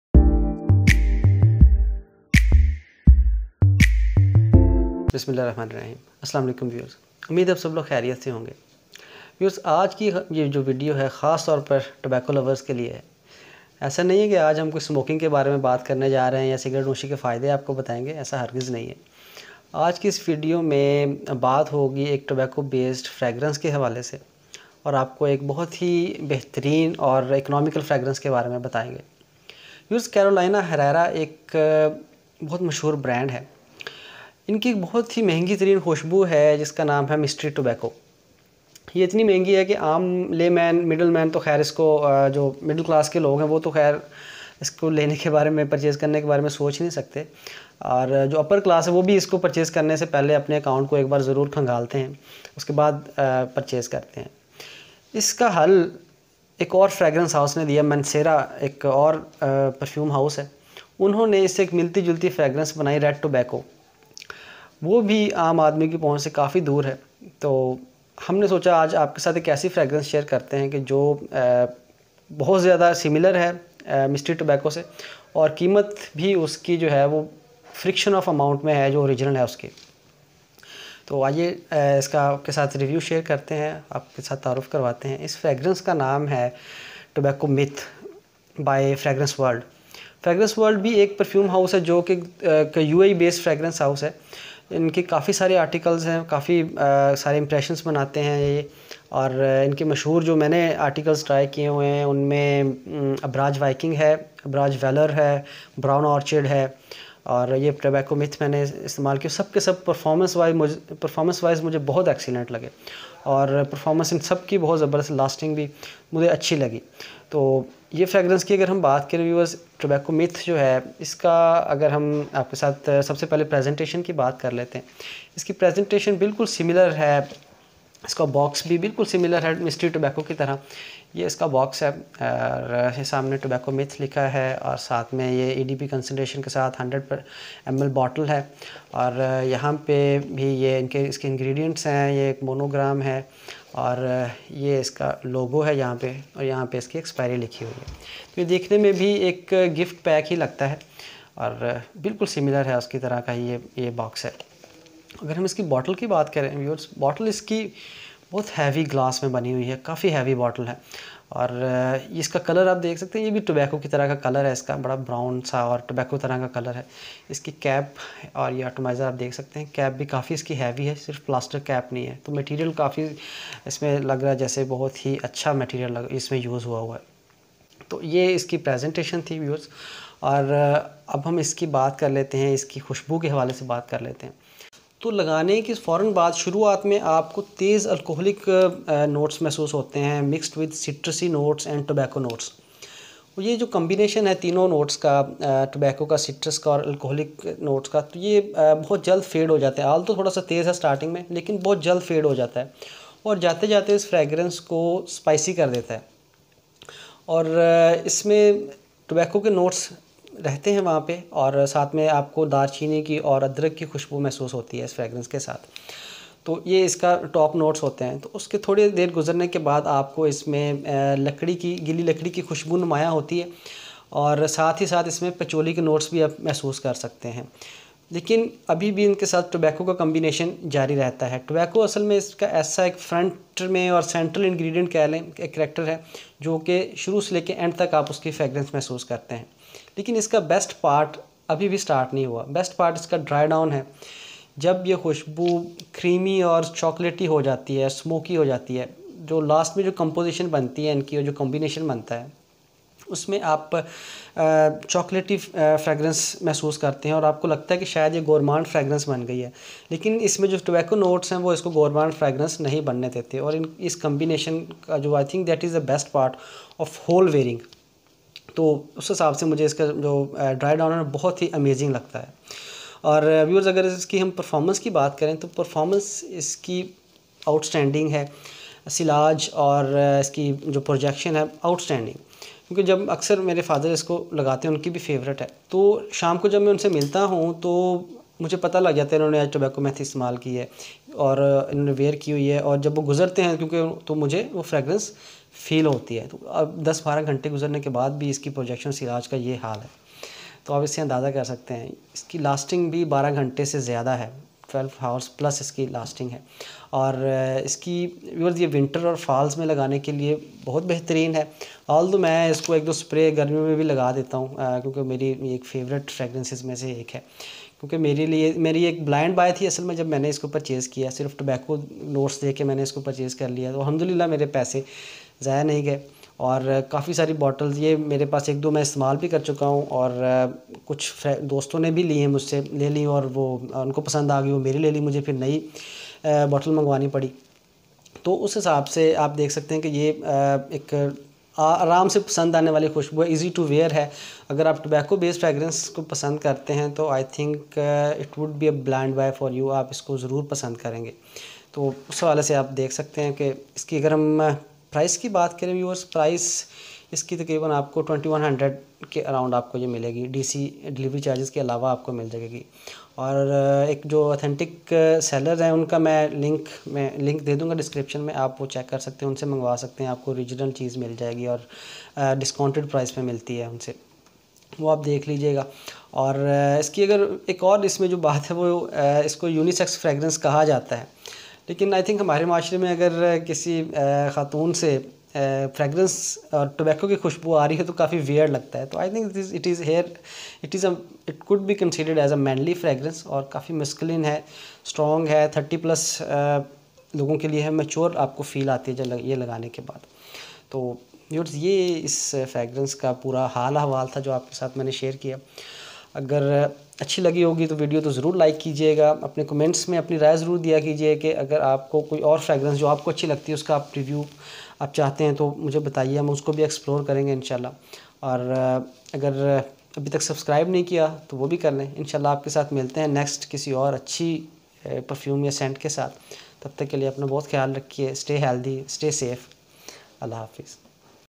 Tobacco Myth by Fragrance World | Best & Budgeted Alternative of CH Mystery Tobacco & Red Tobacco
Talking Art is informed about the best alternative/substitute of Ch Mystery Tobacco and Red Tobacco. Tobacco Myth is budgeted and very similar to CH Mystery Tobacco & Red Tobacco. You can get this perfume in the friction of the original amount of CH Mystery Tobacco.  It is a bitter in first few minutes but become spicy afterwards. most interesting part is its dry down when its become a sweet tobacco fragrance.
Tobacco Myth is best in term of Performance. Projection, Sillage and Lasting, every aspect is excellent. It Lasts more than 12 hrs.
you can get this unique fragrance at excellent price from the following authentic seller. I am sharing his FB profile